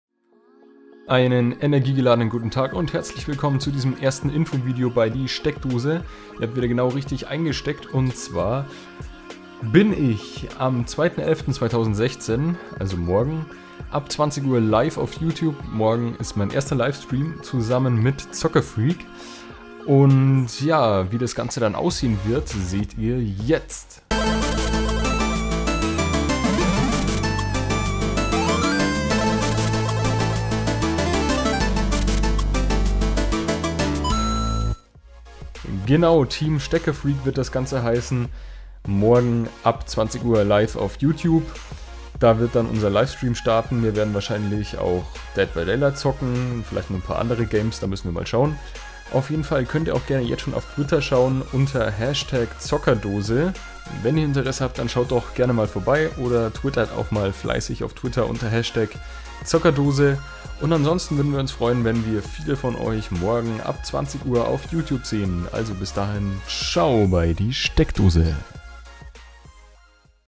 LIVESTREAM-ANKÜNDIGUNG | TeamSteckerfreak | DieSteckdoze
Morgen, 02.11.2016 ab 20 Uhr geht los mit meinem ersten Livestream hier auf YouTube zusammen mit Zckrfrk.
Seid dabei! Alle Tweets zu diesem Livestream findet ihr auf Twitter unter Zckerdose.
Wir sehen uns morgen. Bis dann bei DieSteckdoze!